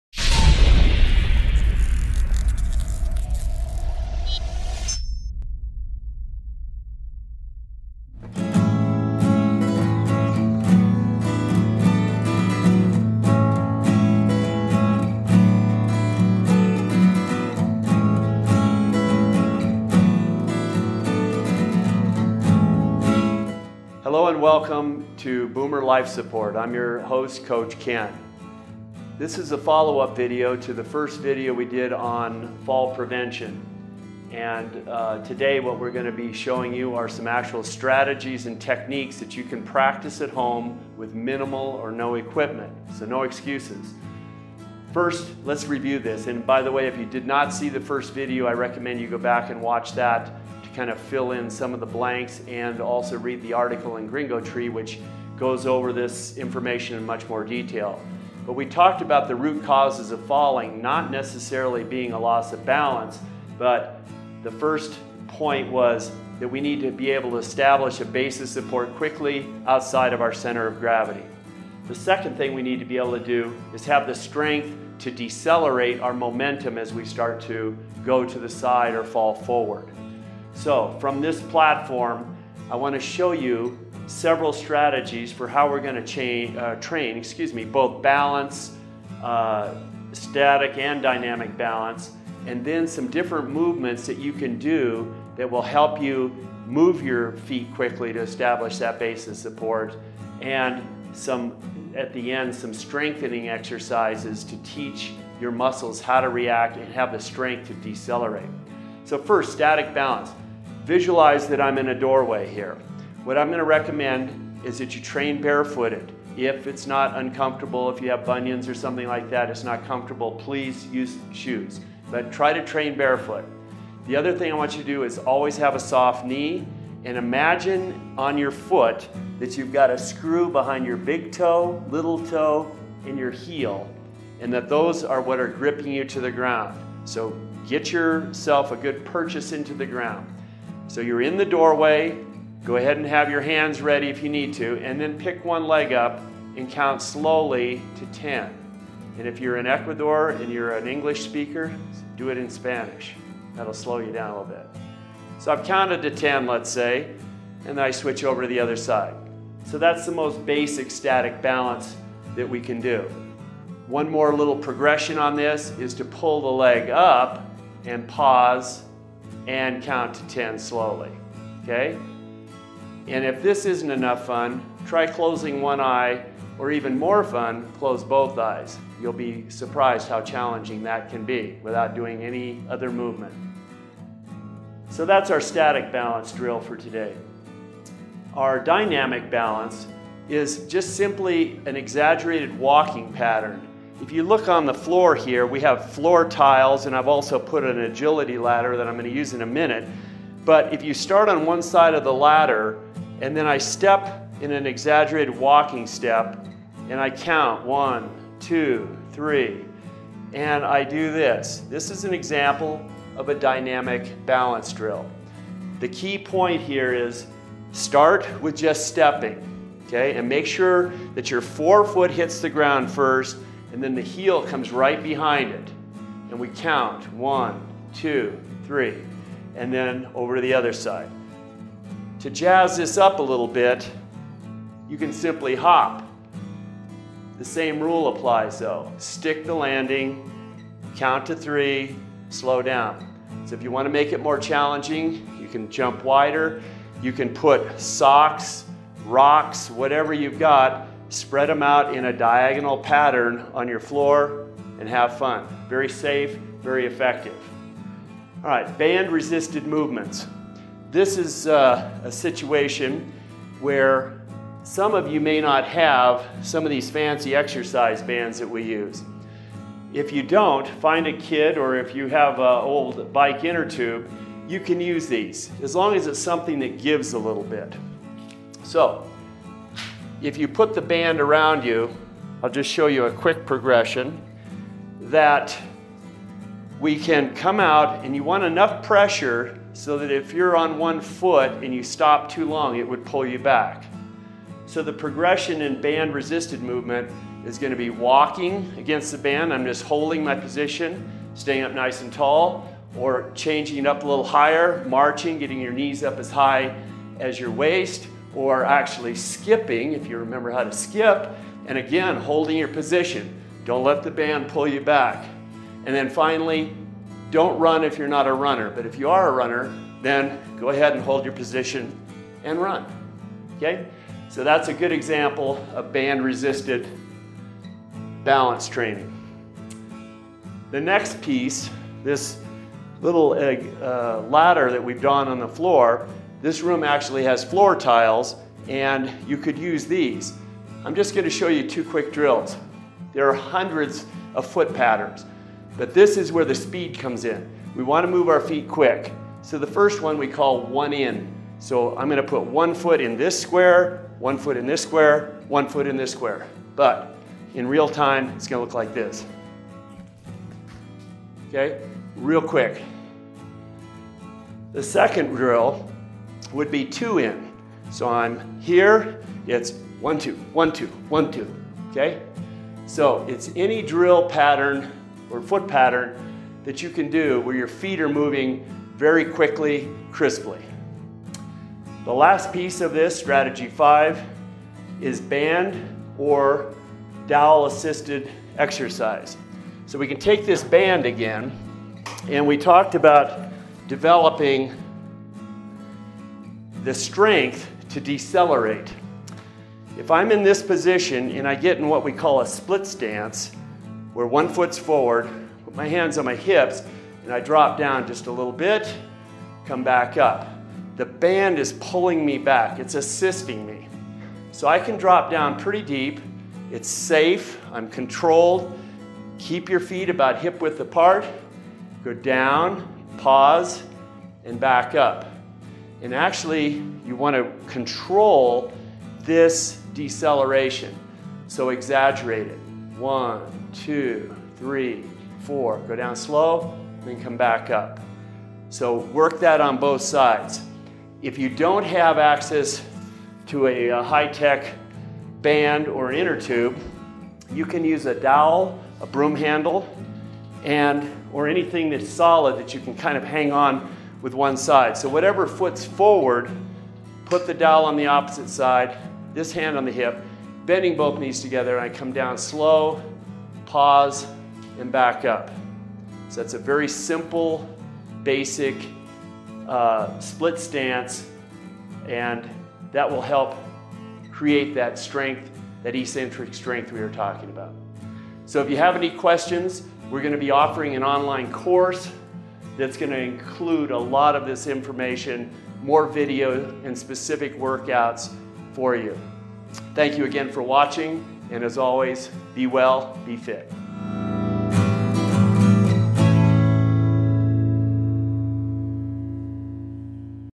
Boomer Life Support: Why We Fall, Part 2
Coach Ken Dobberpuhl picks up where he left off from Part One, teaching baby boomers why they are more prone to fall as they age and sharing some simple exercises that can help.